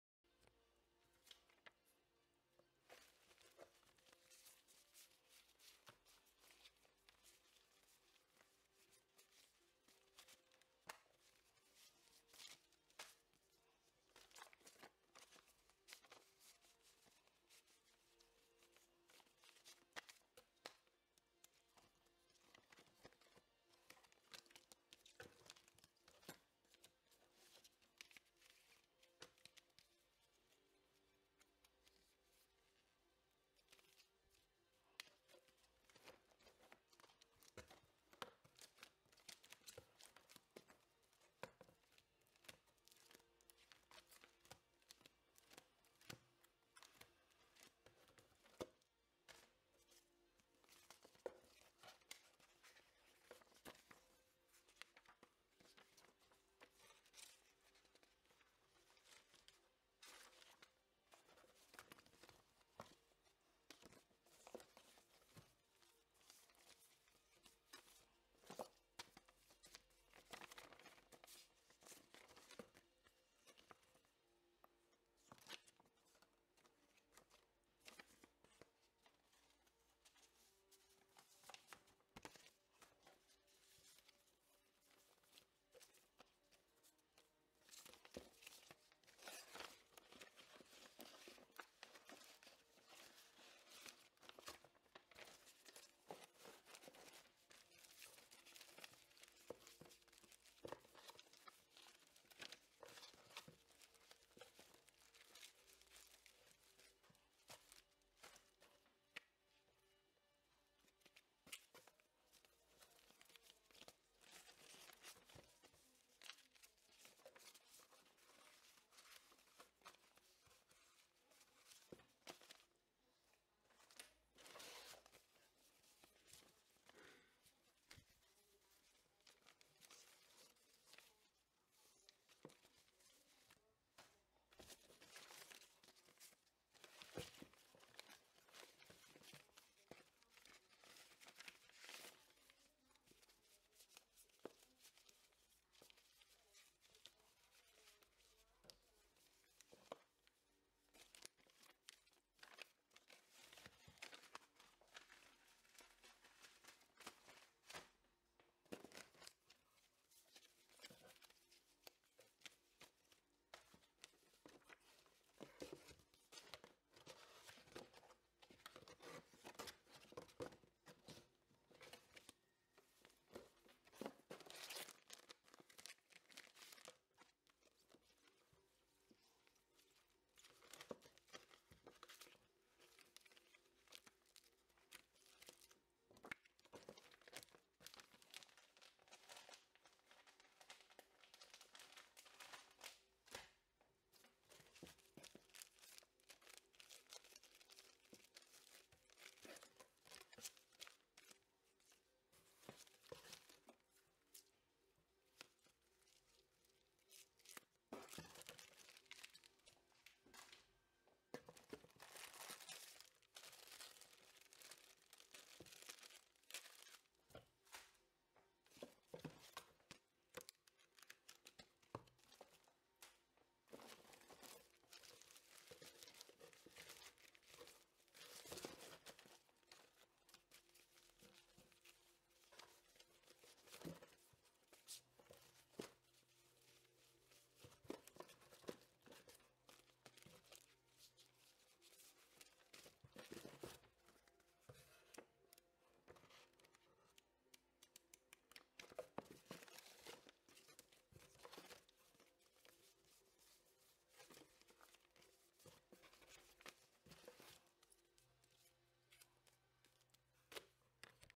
Asta 43 - Lotto 1534 - Lotto divertimento - Miscellanea di alcune centinaia fra francobolli ecc.
Lotto divertimento - Miscellanea di alcune centinaia fra francobolli, lettere, interi postali, figurine e altro, riuniti in una scatola. Insieme molto misto, per specialisti, da esaminare con attenzione.